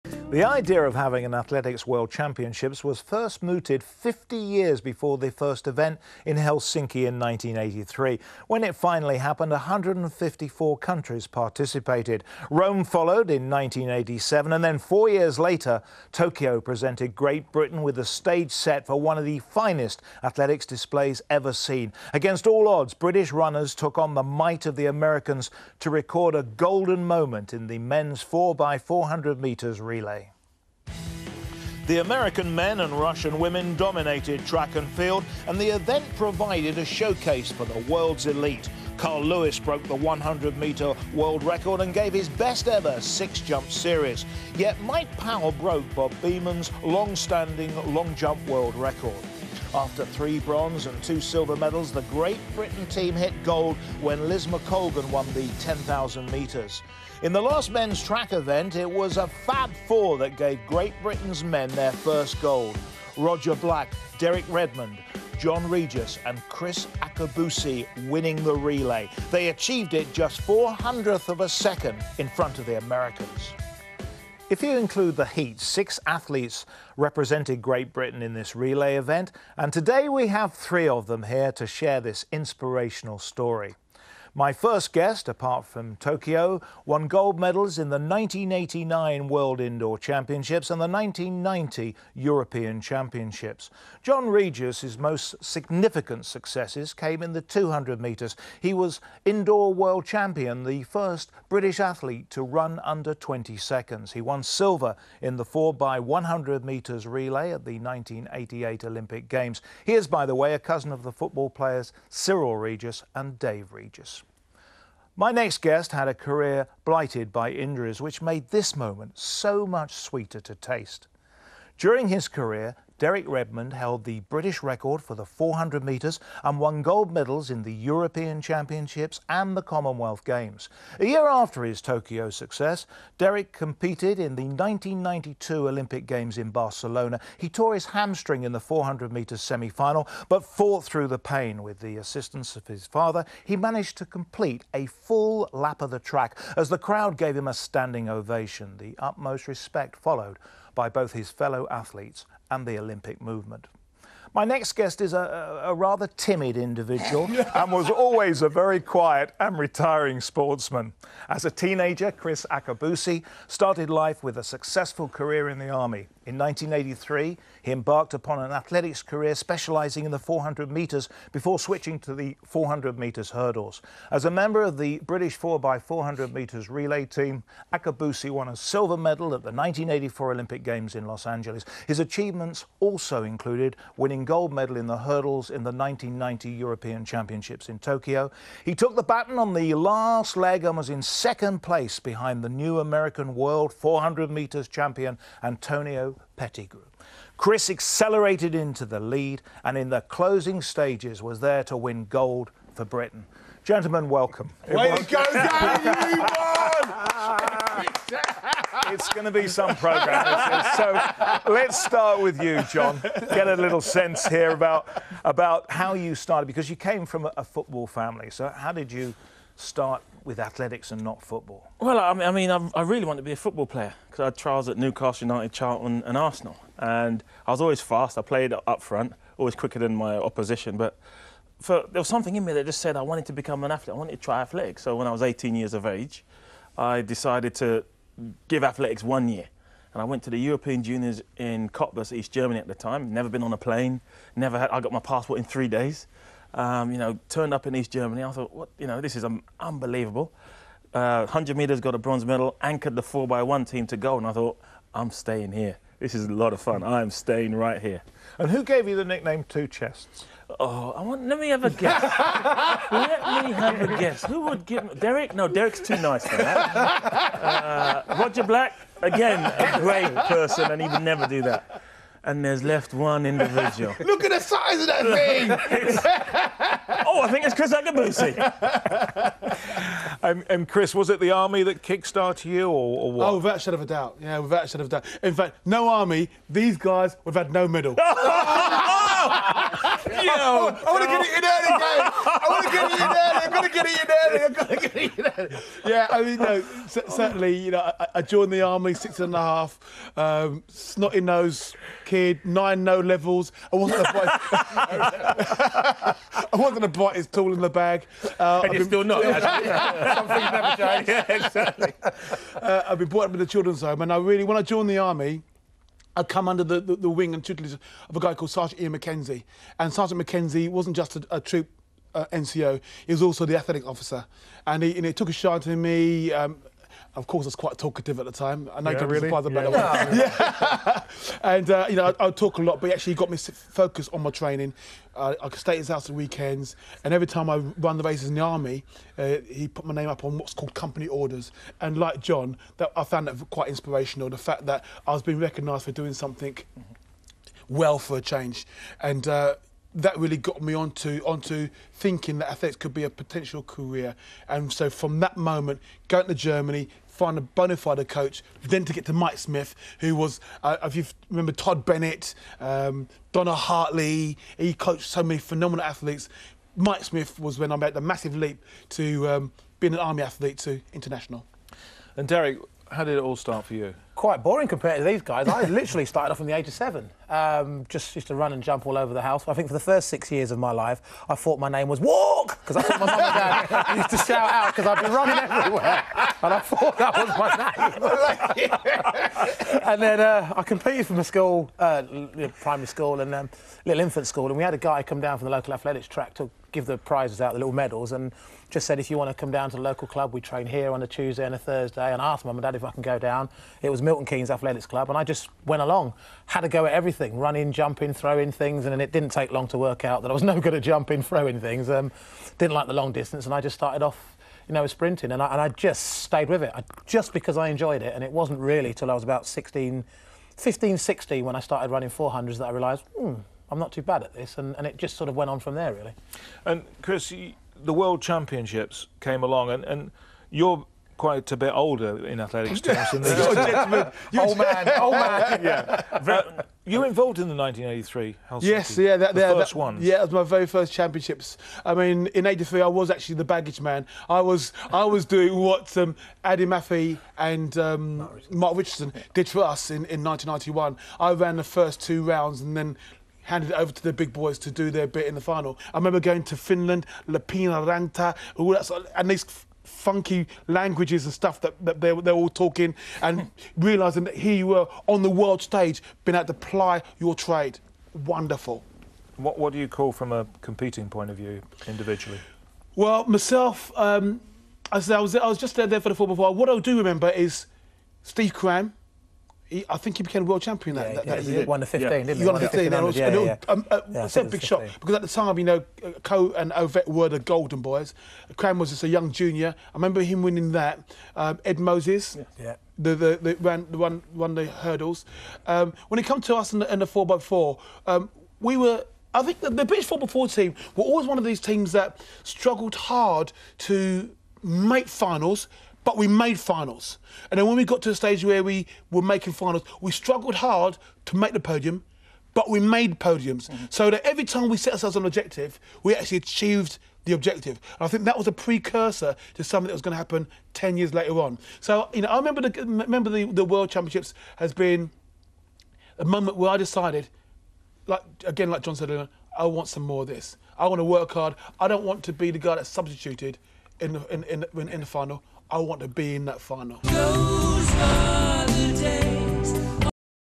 Time of our Lives: 1991 Tokyo 4x400m relay (signed) pt 1 of 4
Three of team GB look back on the infamous relay nearly twenty years on.